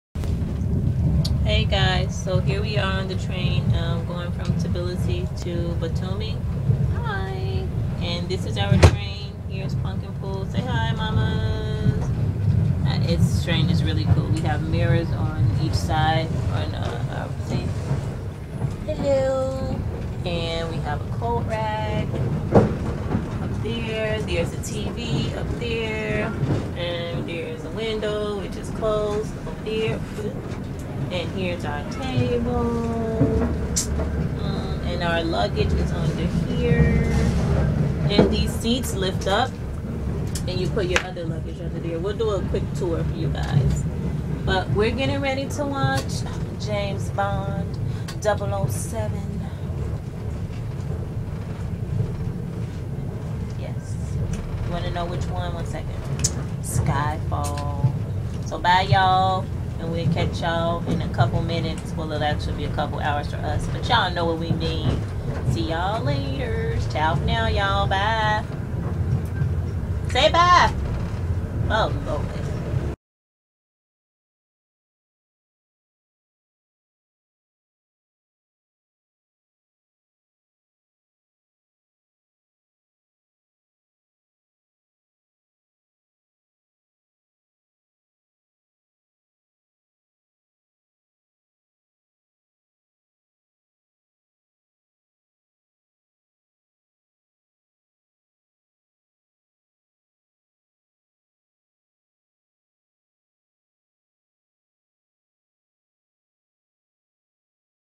TBILISI TO BATUMI: OVERNIGHT GEORGIAN RAILWAYS TRAIN
On our way from Georgia to Turkey, my daughter and I needed to get from Tbilisi to Batumi.  We decided to take the overnight train as it seemed the safest and most conveinent way.  It turned out to be one of our best travel experiences.